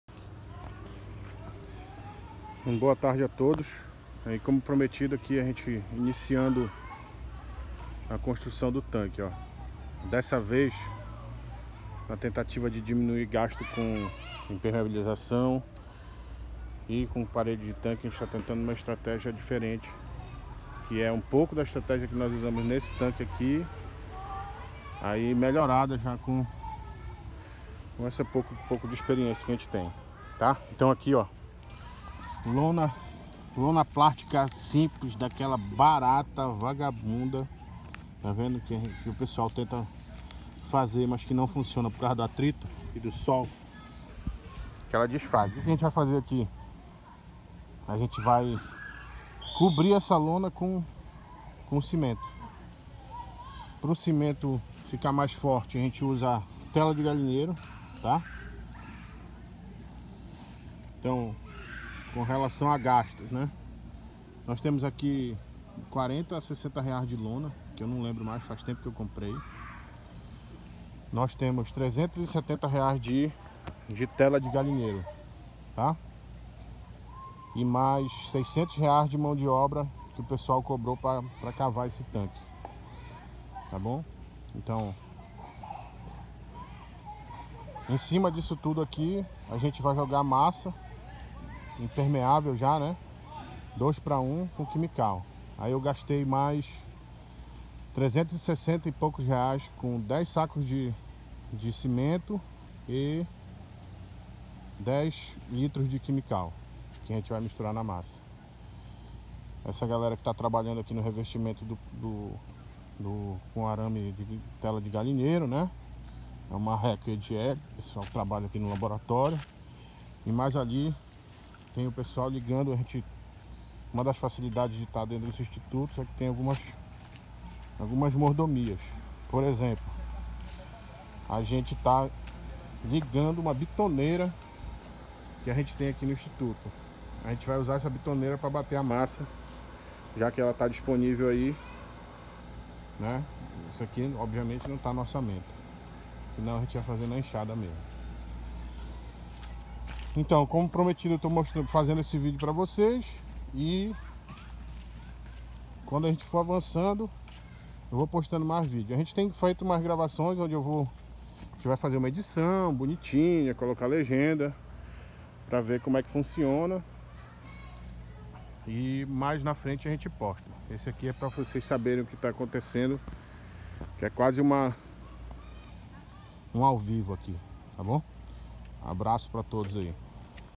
Como Construir Tanque de Aquicultura Adaptado para Exercício Físico: Parte 01
Como construímos o tanque adaptado para exercício físico dos tambaquis.
Orçamento Inicial:

R$ 600,00 Escavação
R$ 373,00 Rolo de Tela Hexagonal de Galinheiro
R$ 334,80,00 10 Sacos de Cimento e 10 Litros de Quimikal